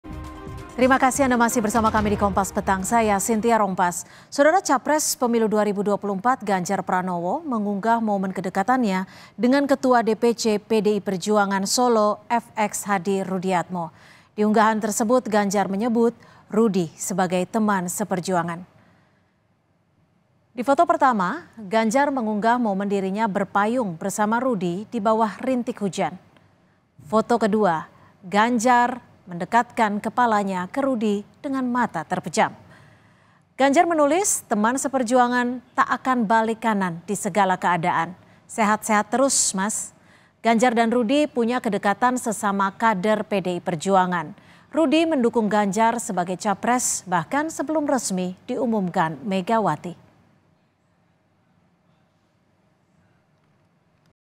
Ganjar Unggah Momen Kedekatan dengan Ketua DPC PDIP Solo FX Hadi Rudyatmo: Teman Seperjuangan
KOMPAS.TV - Ganjar Unggah Momen Kedekatan dengan Ketua DPC PDIP Solo FX Hadi Rudyatmo: Teman Seperjuangan

Kompas TV
Independen | Terpercaya